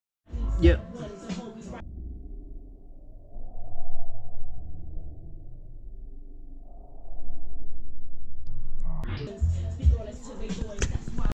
Gopro Hero 6 Black Rusty slowmo
Gopro hero 6 Black 240fps 1920x1080
Cat drop test.